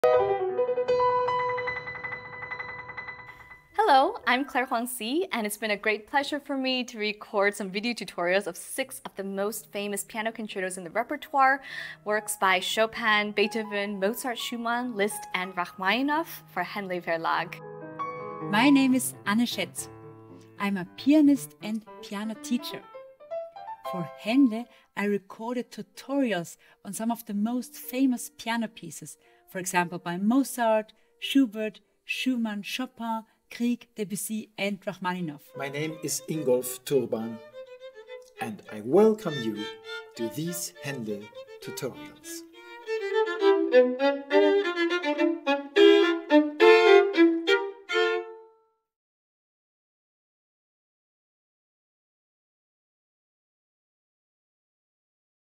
Henle Masterclass - Meet the Faculty
Meet the Henle Masterclass Faculty: Claire Huangci, acclaimed pianist and professor at Musikhochschule Weimar, the renowned Munich based piano pedagogue Anne Schätz and Ingolf Turban, violin virtuoso and passionate teacher. Meet them all on our brand-new video tutorial platform and learn from their very own professional and pedagogical experience. And stay tuned: during the next months, we will add many more tutorials with further musicians and for more instruments!